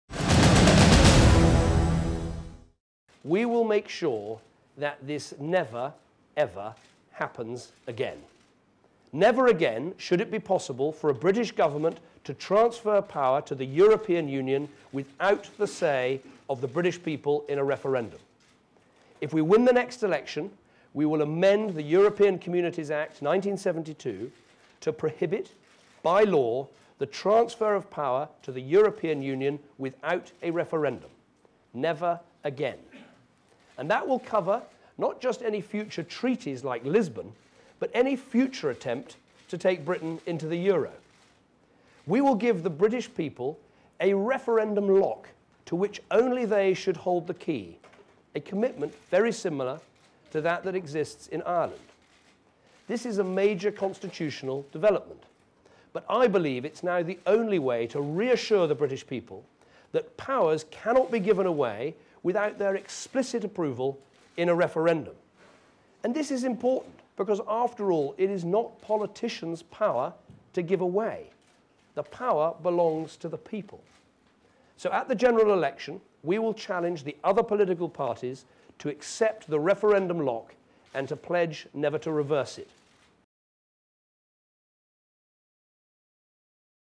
David Cameron promises vote on future EU promises
The Tory leader has said the party would change the law so no further powers could be transferred to Brussels.